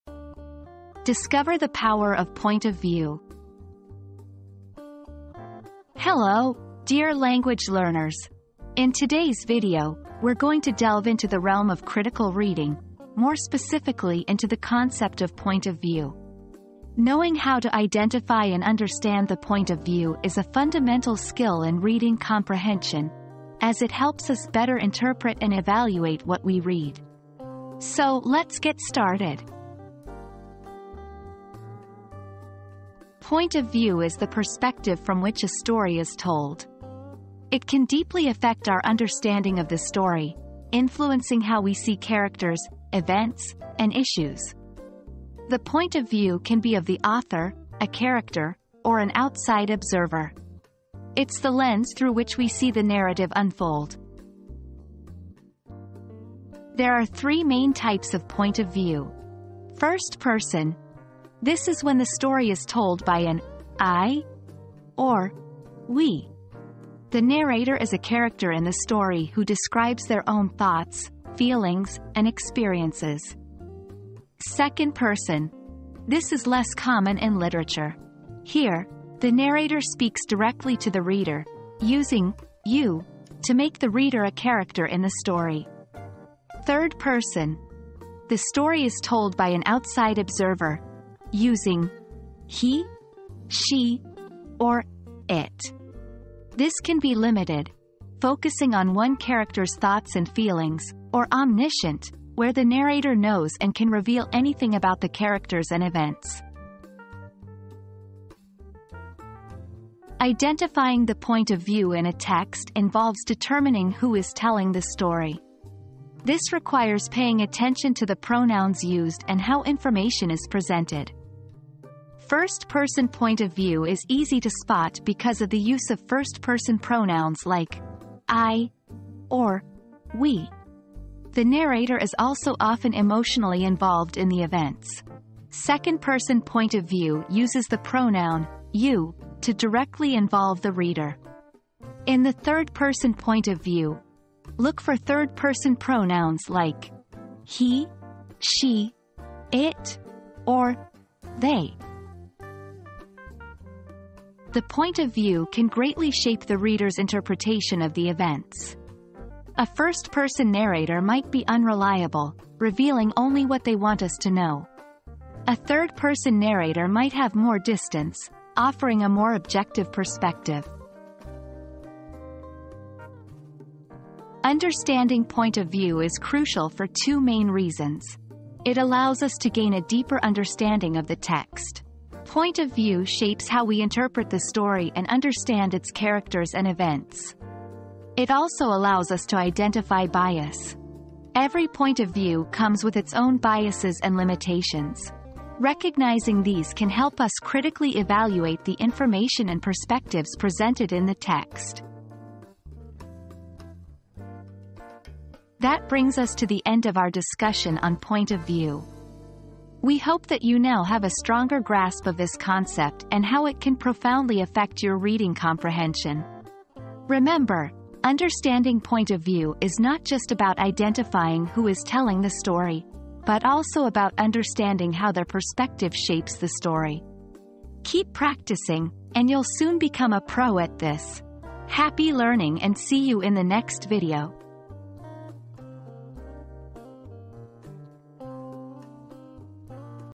Discover the Power of Point of View
Discover the Power of Point of View • Uncover the secrets of critical reading and enhance your comprehension skills by understanding the concept of point of view. Learn how different perspectives can shape our understanding of characters, events, and issues. Explore the three main types of point of view and discover how to identify them in texts. Get ready to see narratives in a whole new light!

00:00 • Introduction - Discover the Power of Point of View
00:32 • What is Point of View?
01:49 • Identifying Point of View
02:53 • The Importance of Point of View